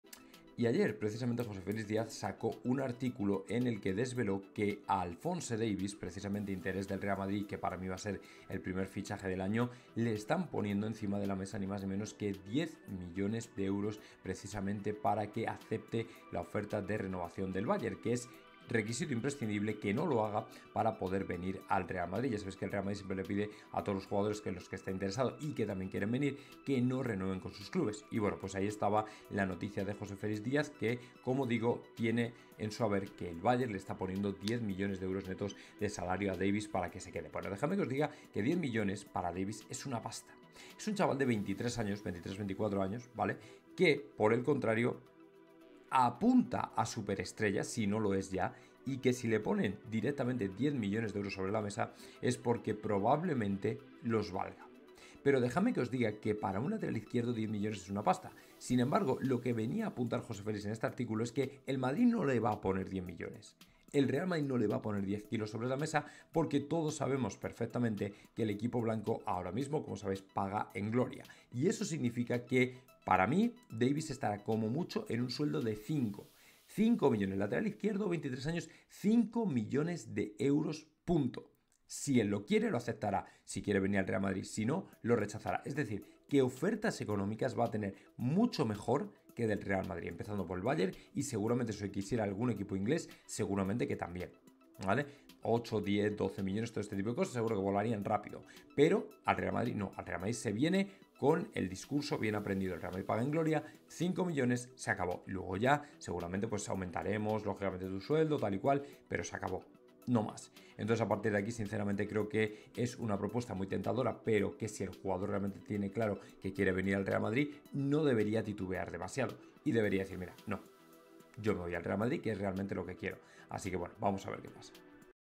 ¡LE PONEN 10 MILL! | JFD desvela el crack que "tiene" 10M para renovar ¡dice NO para venir al R.M!
¡LE PONEN 10 MILLONES!  |  JFD desvela el crack que "tiene" 10M para renovar...¡y dice que NO para venir al R.Madrid!

CÓDIGO: FANBW30

jotarealmadrid

jotarealmadrid

Dr_jota_163

Doctorjota